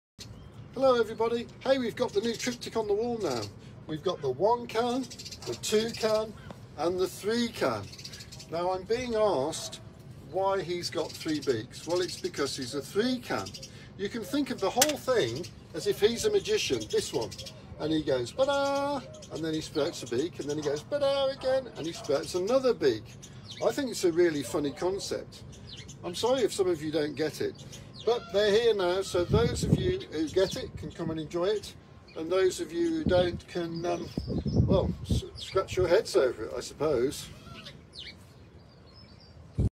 Martin Cheek's mosaic: The ‘Toucan Triptych’ displayed on his studio wall:
In this short film, Martin shows the ‘Toucan Triptych’ displayed on his studio wall: